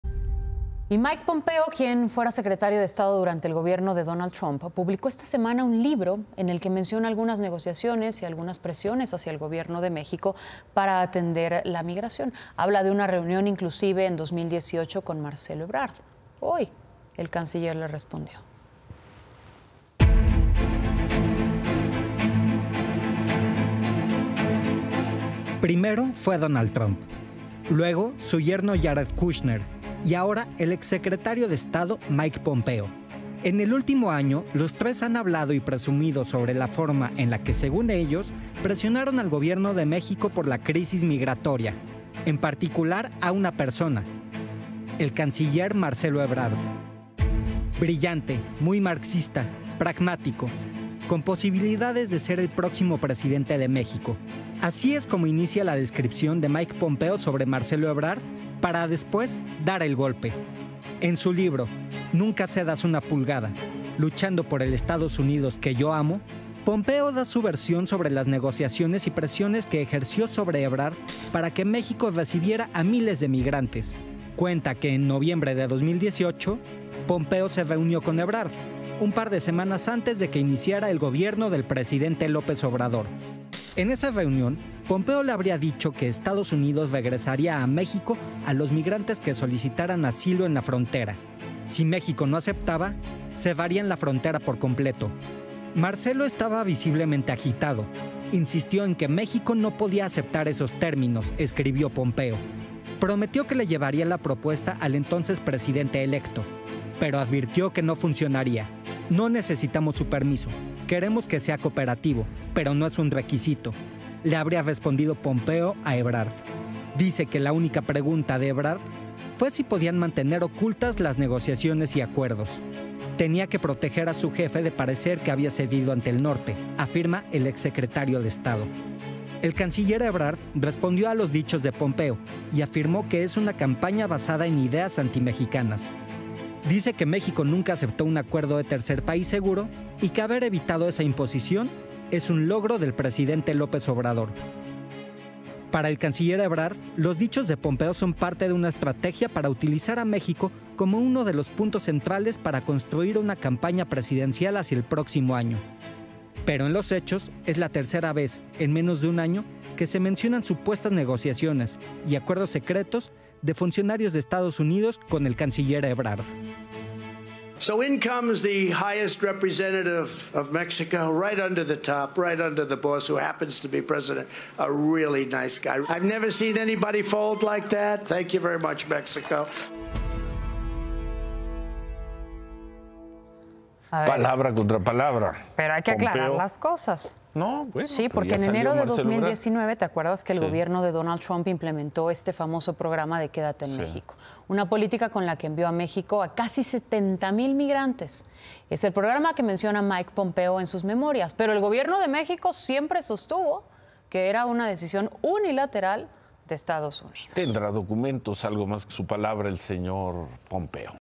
Ebrard habría pedido que se ocultara polémico trato migratorio con EU | Ciro Gómez Leyva
CiroEnImagen EU sigue presumiendo la forma en la que presionaron al gobierno mexicano por la crisis migratoria. A través de un libro, el ex secretario de Estado, MikePompeo, dio su versión sobre las negociaciones y presiones que ejerció sobre MarceloEbrard para que México recibiera a miles de migrantes